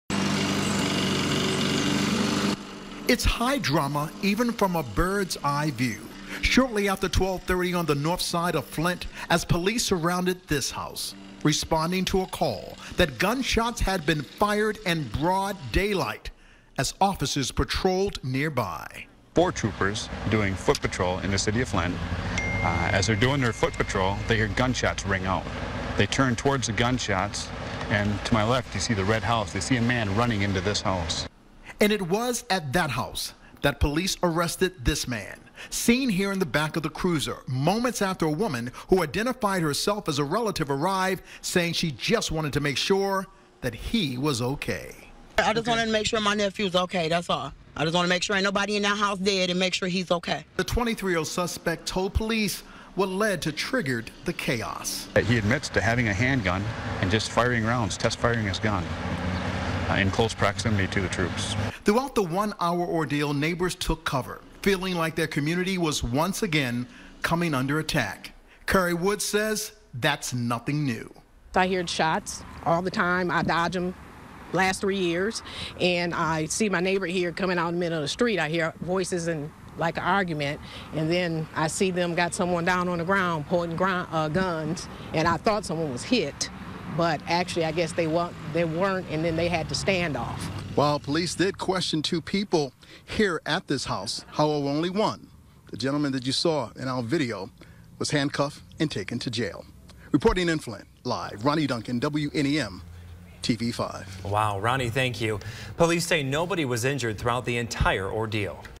Man fires Gun near Police
Flint New Gun Owner picks Wrong time to fire off in Neighborhood.